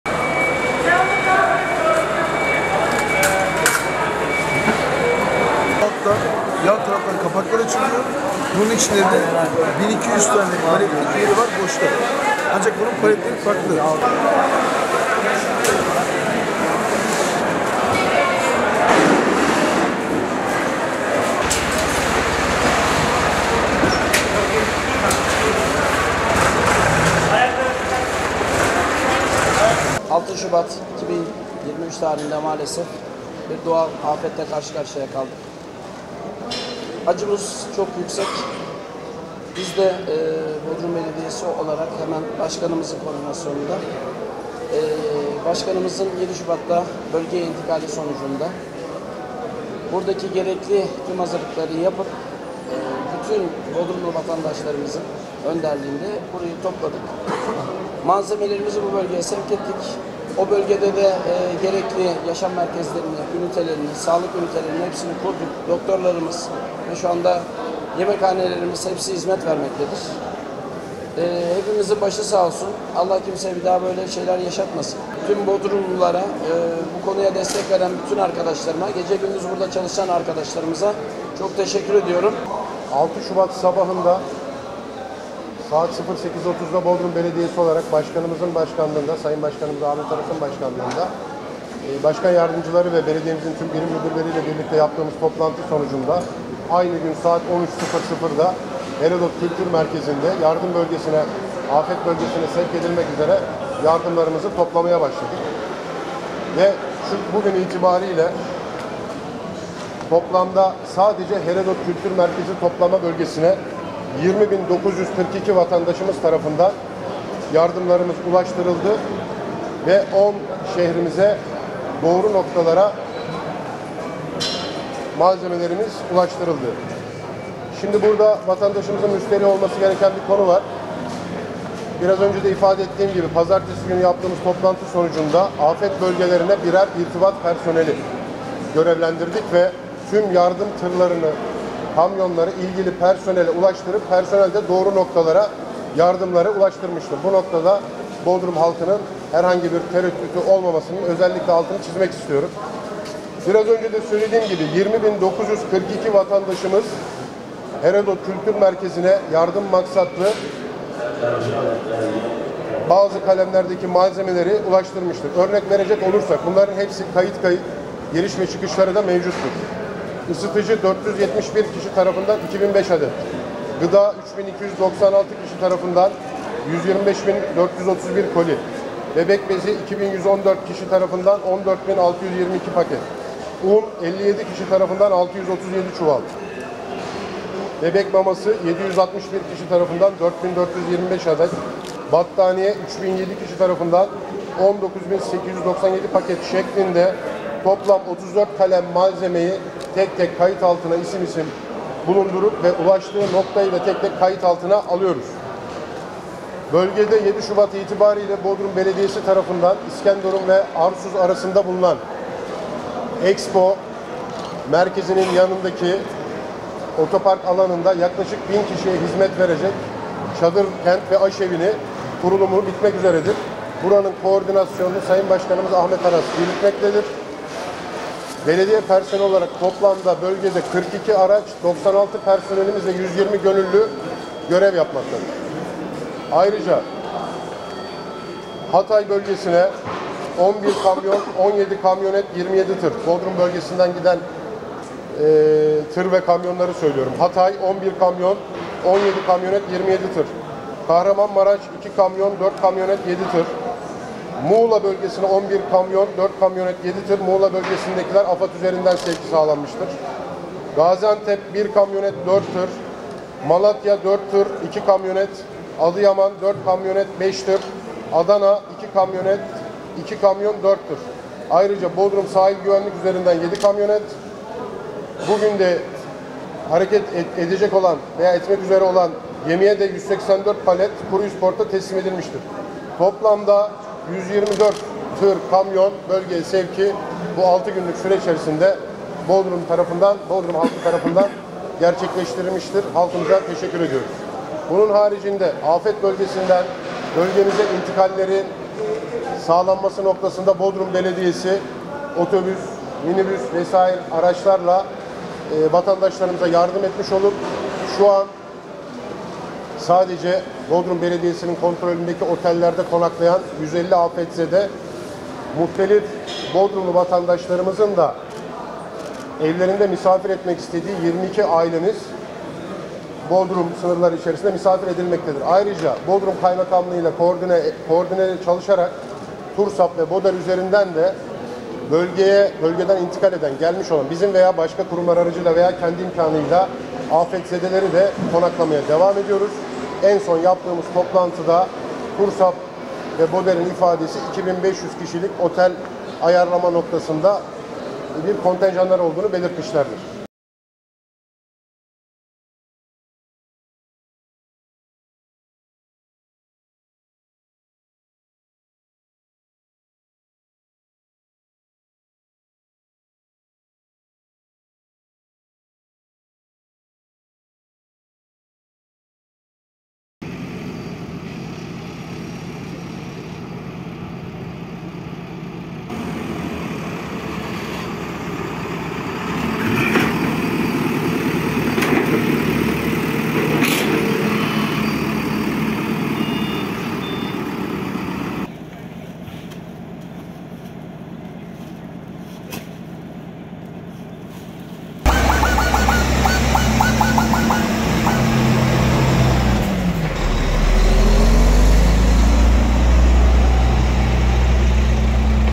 Bodrum Belediyesi'nden deprem yardımları bilgilendirme toplantısı, 12 Şubat 2023
Bodrum Afet Koordinasyon Merkezi’nde gerçekleştirilen toplantıya Bodrum Belediyesi Başkan Vekili İbrahim Akbaş, Bodrum Belediyesi Başkan Yardımcısı Tayfun Yılmaz ve ilgili birim müdürleri katıldı. Açıklamada yapılan yardımlar ve çalışmalar ile ilgili son durum aktarıldı.